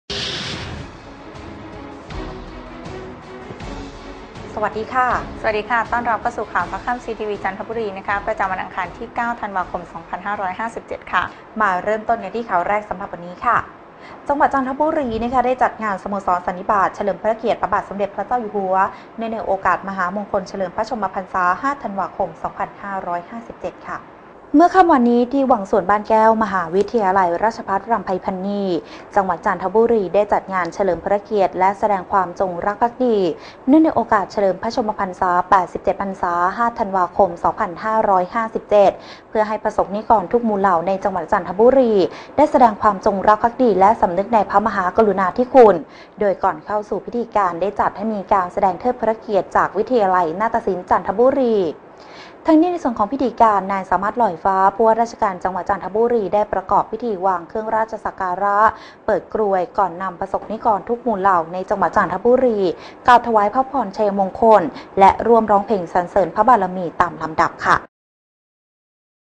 9-12-57 จ.จันทบุรีจัดงานสโมสรสันนิบาตเฉลิมพระเกียรติ
จังหวัดจันทบุรีจัดงานสโมสรสันนิบาตเฉลิมพระเกียรติพระบาทสมเด็จพระเจ้าอยู่หัวเนื่องในโอกาส มหามงคลเฉลิมพระชนมพรรษา 5 ธันวาคม 2557